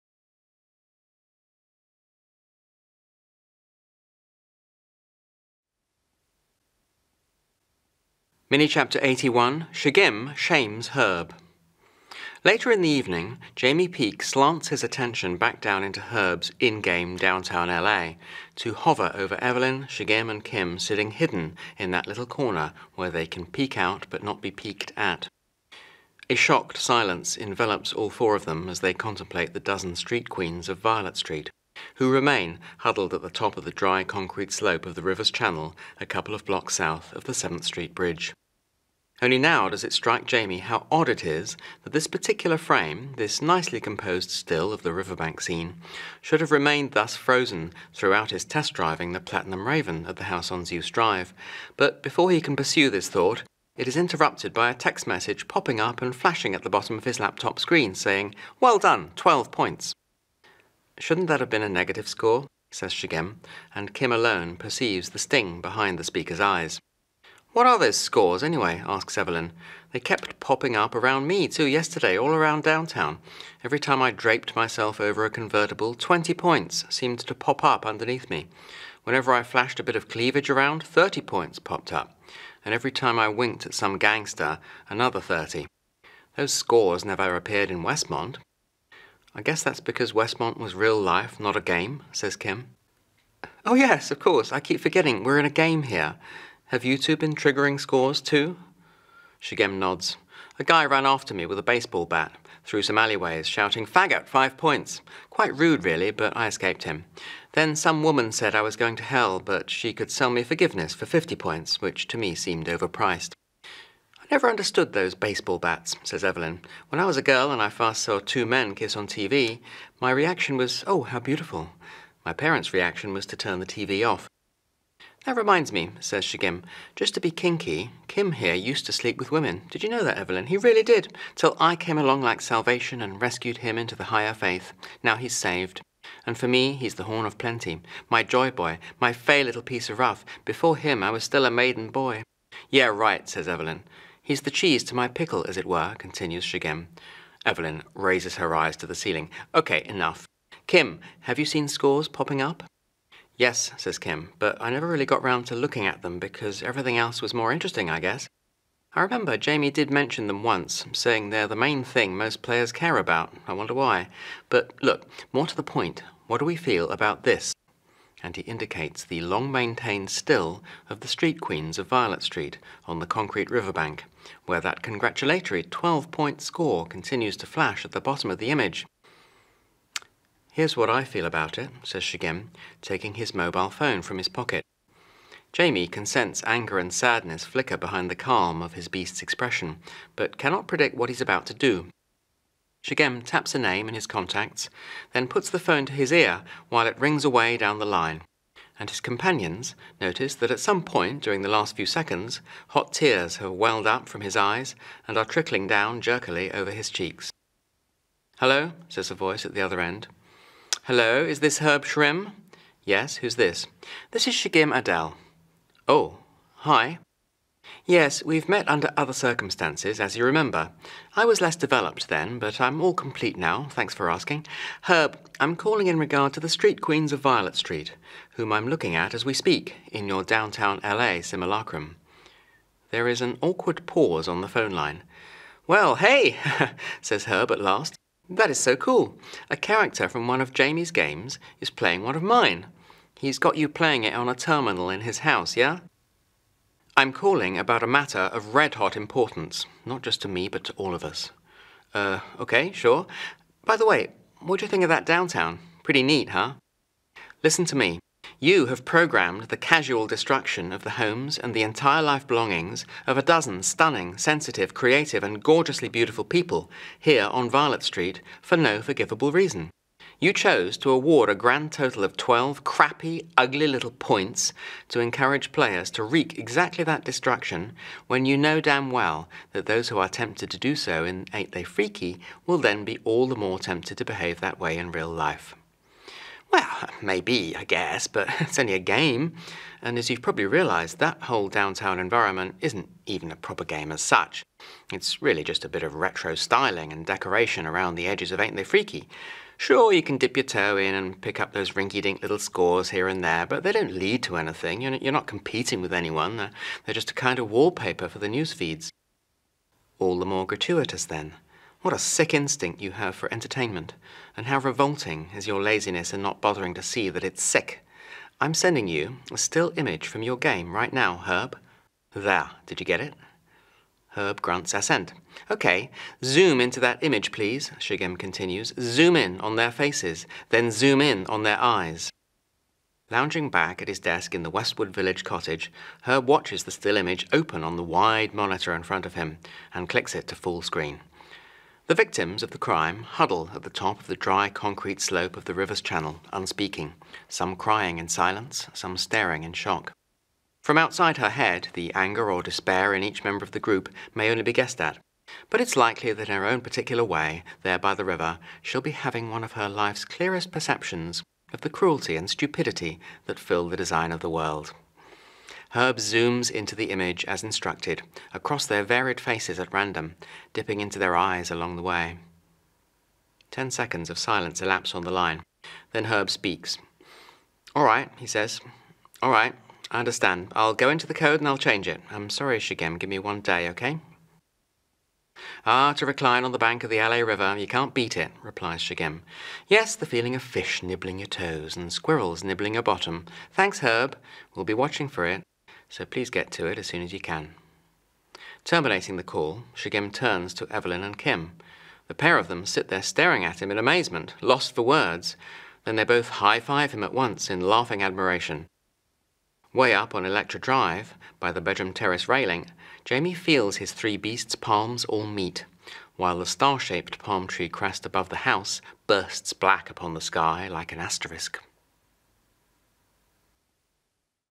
Rohan Quine, “The Beasts of Electra Drive”, 81 Shigem shames Herb
Later in the evening, Jaymi Peek slants his attention back down into Herb’s in-game Downtown L.A., to hover over Evelyn, Shigem and Kim sitting hidden in that little corner where they can peek out but not be peeked at.

 A shocked silence envelops all four of them as they contemplate the dozen street queens of Violet Street, who remain huddled at the top of the dry concrete slope of the River’s channel a couple of blocks south of the Seventh Street bridge.

 Only now does it strike Jaymi how odd it is that this particular frame—this nicely-composed still of the riverbank scene—should have remained thus frozen throughout his test-driving the Platinum Raven at the house on Zeus Drive. But before he can pursue this thought, it is interrupted by a text message popping up and flashing at the bottom of his laptop screen saying “Well done, 12 points!”.

 “That reminds me,” says Shigem. “Just to be kinky, Kim here used to sleep with women. Did you know that, Evelyn? He really did. Till I came along like salvation, and rescued him into the higher faith. Now he’s saved. And for me, he’s the horn of plenty. My joy-boy, my fey little piece of rough. Before him, I was still a maiden boy.”

 “Yeah, right,” says Evelyn.

 “He’s the cheese to my pickle, as it were,” continues Shigem.

 Evelyn raises her eyes to the ceiling. “OK, enough. Kim, have you seen scores popping up?”

 “Yes,” says Kim. “But I never really got round to looking at them, because everything else was more interesting, I guess. I remember Jaymi did mention them once, saying they’re the main thing most players care about; I wonder why? But look—more to the point, what do we feel about this?” and he indicates the long-maintained still of the street queens of Violet Street on the concrete river-bank, where that congratulatory 12-point score continues to flash at the bottom of the image.

 “Here’s what I feel about it,” says Shigem, taking his mobile phone from his pocket. Jaymi can sense anger and sadness flicker behind the calm of his Beast’s expression, but cannot predict what he’s about to do. Shigem taps a name in his Contacts, then puts the phone to his ear while it rings away down the line; and his companions notice that at some point during the last few seconds, hot tears have welled up from his eyes and are trickling down jerkily over his cheeks.

 “Hallo?” says a voice at the other end.

 “Hallo, is this Herb Shrim?”

 “Yes. Who’s this?”

 “This is Shigem Adele.”

 “Oh. Hi.”

 “Yes, we’ve met under other circumstances, as you remember. I was less developed then, but I’m all complete now, thanks for asking. Herb, I’m calling in regard to the street queens of Violet Street, whom I’m looking at as we speak, in your Downtown L.A. simulacrum.”

 There is an awkward pause on the phone line. “Well, hey,” says Herb at last, “that is so cool. A character from one of Jaymi’s games is playing one of mine! He’s got you playing it on a terminal in his house, yeah?”

 “I’m calling about a matter of red-hot importance, not just to me but to all of us.”

 “Er, OK, sure. By the way, what d’you think of that Downtown? Pretty neat, huh?”

 Terminating the call, Shigem turns to Evelyn and Kim. The pair of them sit there staring at him in amazement, lost for words; then they both high-five him at once, in laughing admiration.

 Way up on Electra Drive, by the bedroom terrace railing, Jaymi feels his three Beasts’ palms all meet; while the star-shaped palm tree crest above the house bursts black upon the sky, like an asterisk.

And to pick it up from whichever retailer you prefer, the retailers’ links for the audiobook format are at